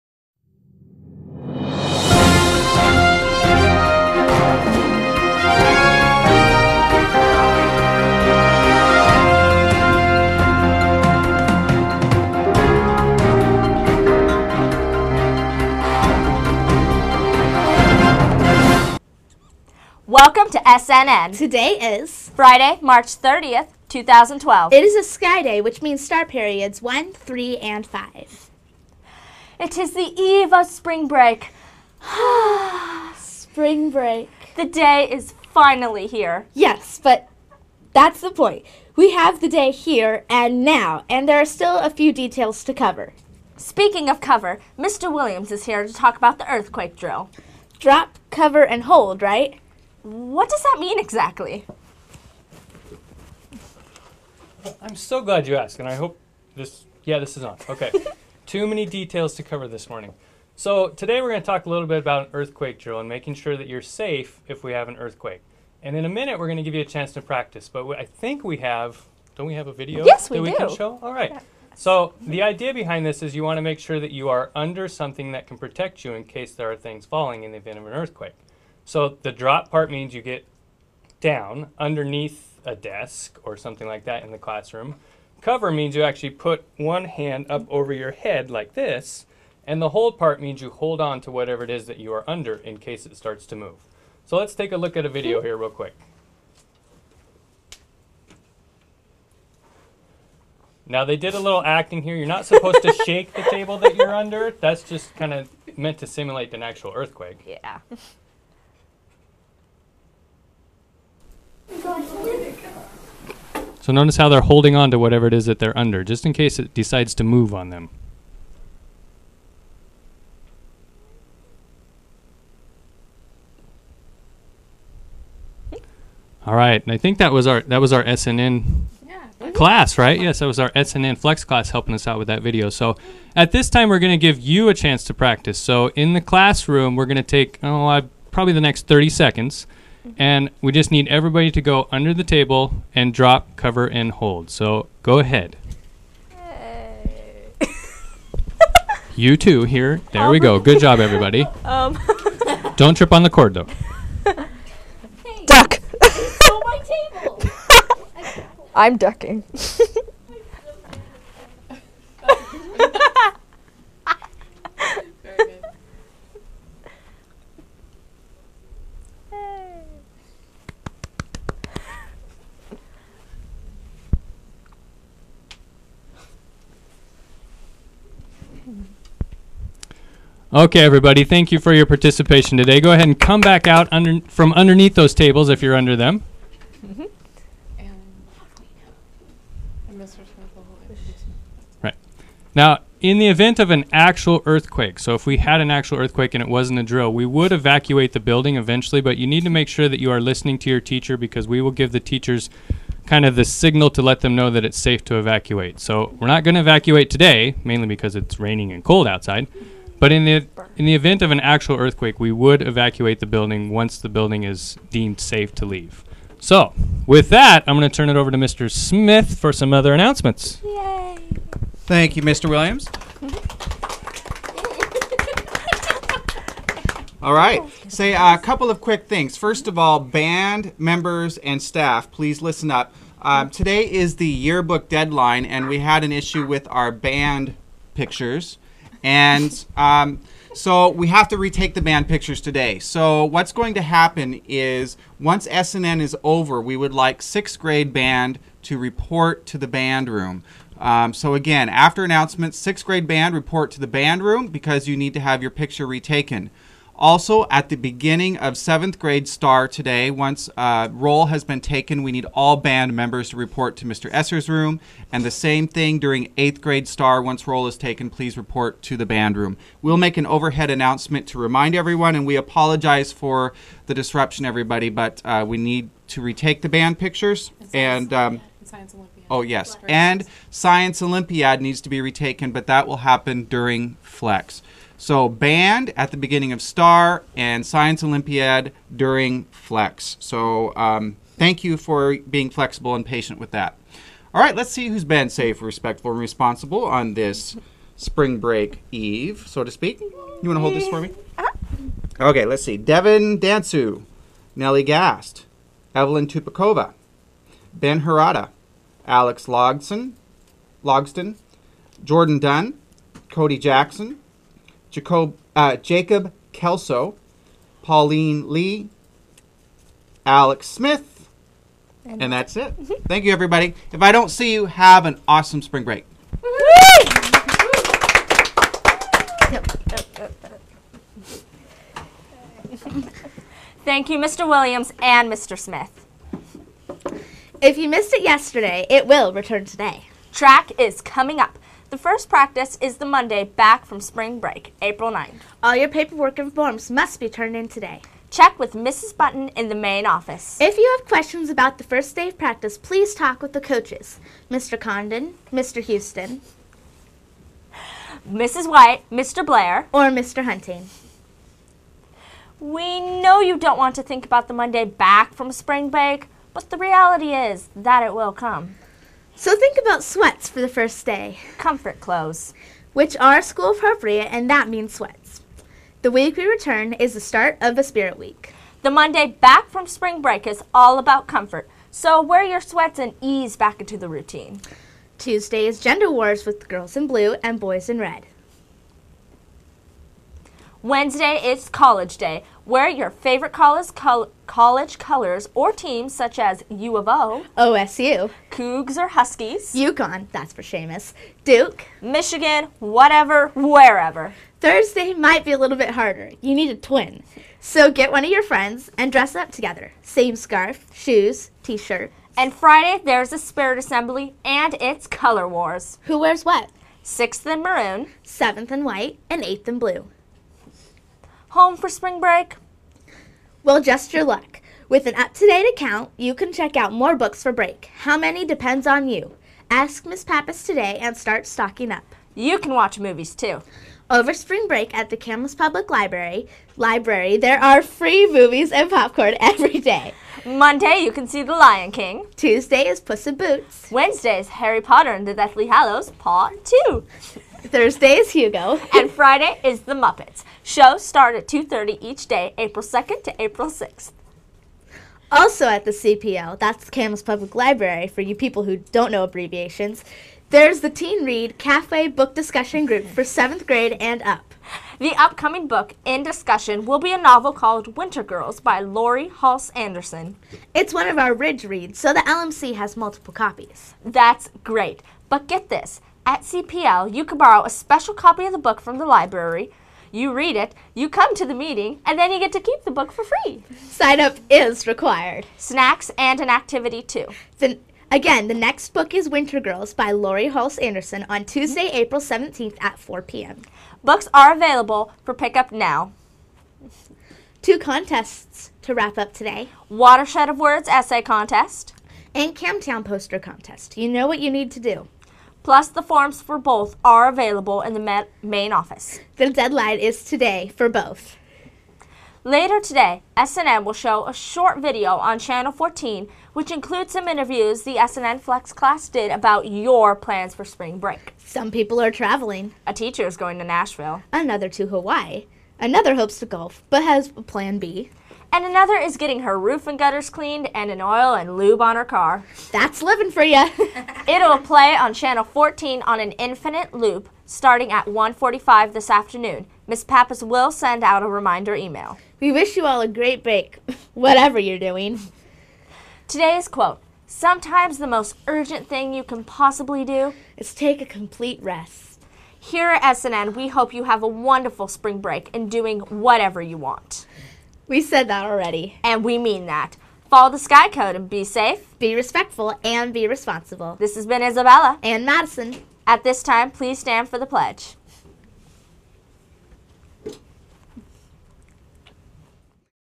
Skyridge Network News (SNN) 3-30-12
Isabella and Madison share some news. Mr. Williams covers our drills from earthquakes and Mr. Smith gives away some Smencils and wishes everyone a happy, restful break.